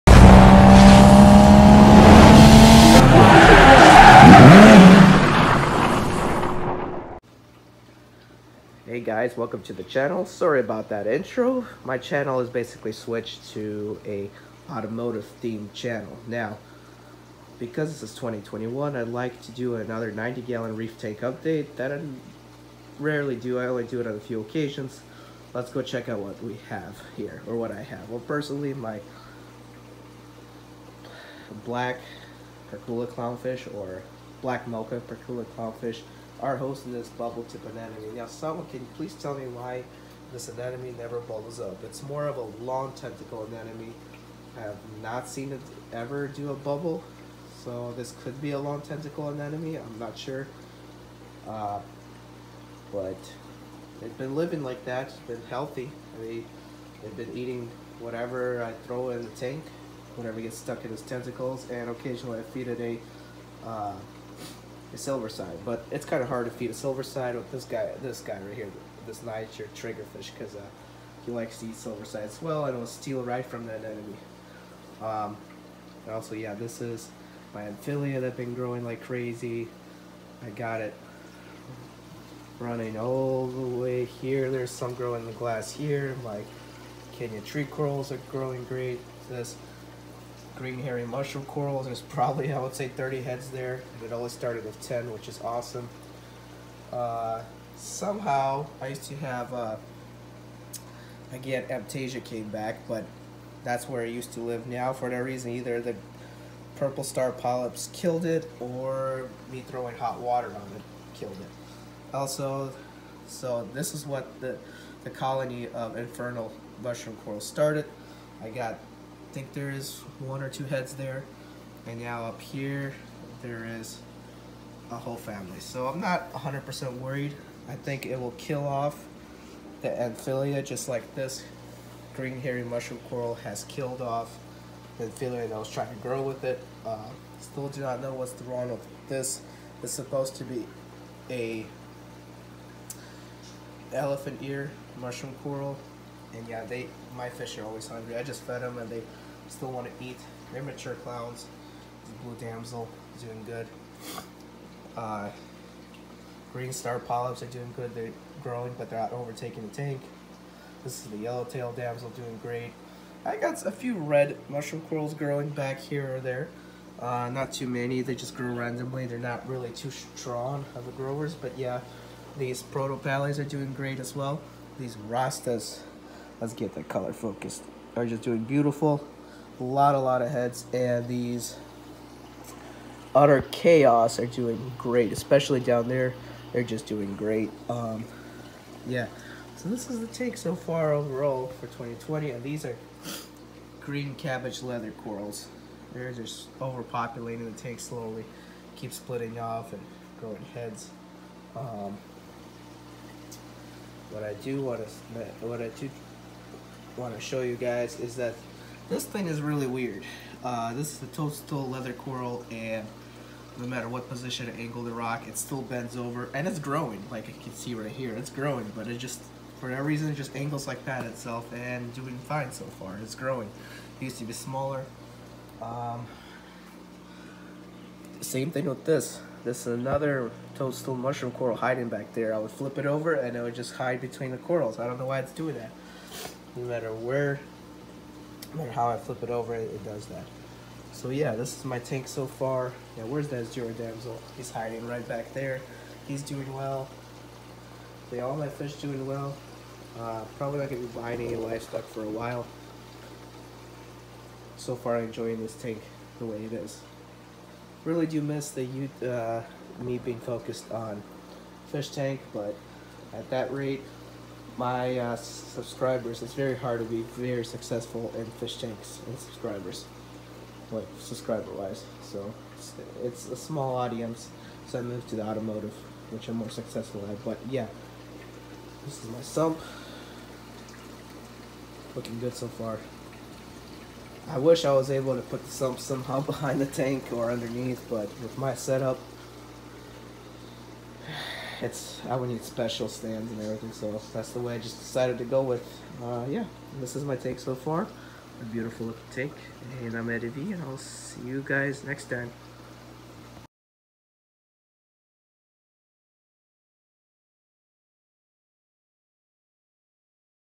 90 Gallon Reef Tank Update January 2021
Here is my fish tank that’s my hobby outside of the automotive hobby.

UCAFU2KnXqDRKsZQ2bDPfn3A?sub_confirmation=1